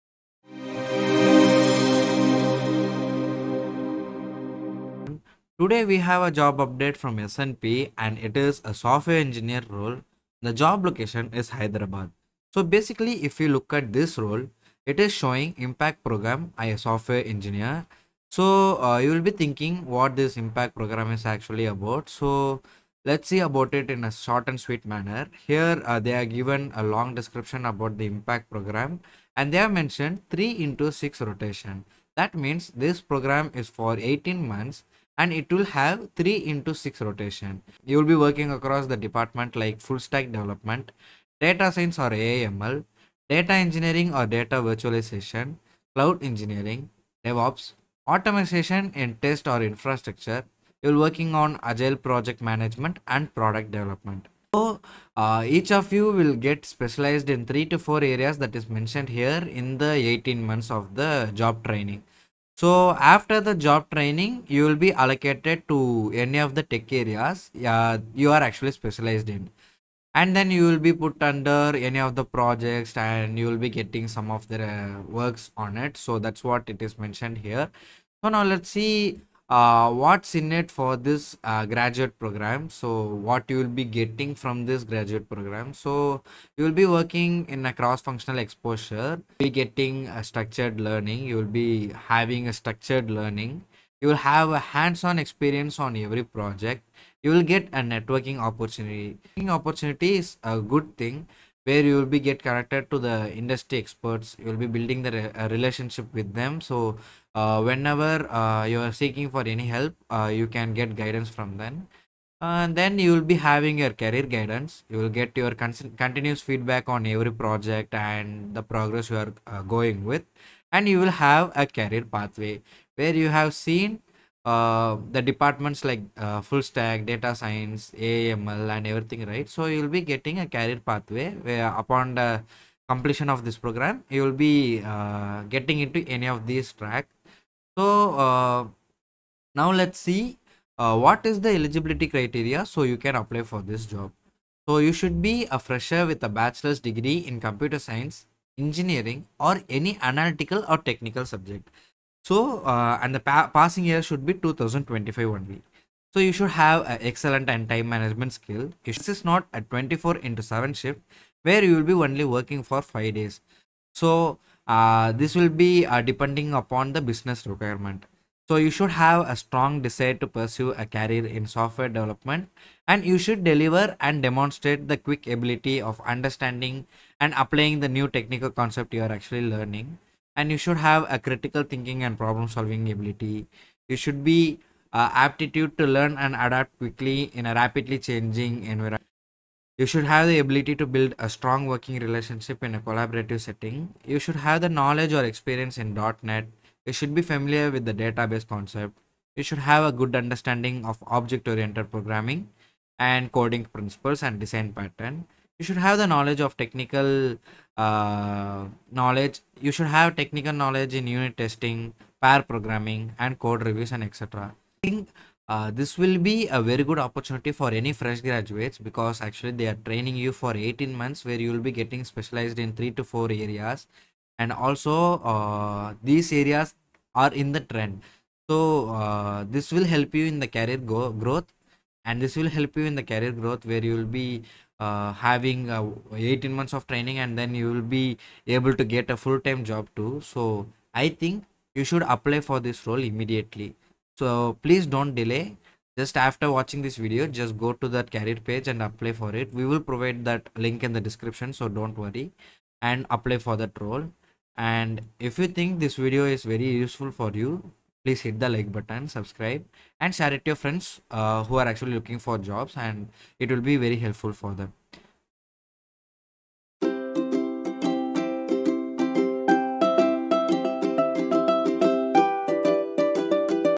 FREE Training Program at S&P Global | Software Engineer | Freshers Guide to IT Careers 2024
Looking to start your career in software engineering? Discover the IMPACT Program at S&P Global—a unique 18-month rotational program designed for fresh graduates to gain hands-on experience in Full Stack Development, Data Science, Cloud Engineering, and more. This video will guide you through the program structure, benefits, and how to apply for this incredible opportunity in Hyderabad, India.